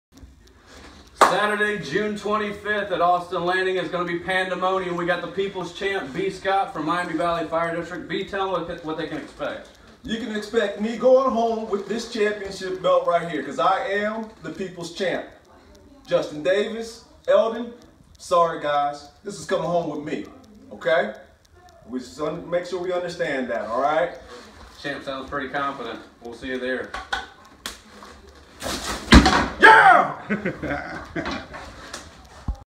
Hunks and Ladders Firefighter acting like The Rock 2 raise $ for charity @ Austin Landing 06/25/2016
A little message to some of the other guys coming straight from The 2016 People's Champ B. Scott from Miami Valley Fire District. Who's the next Hunks and Ladders Champion???
Hunks and Ladders Firefighter Combat Challenge
Saturday June 25th @ The Park at Austin Landing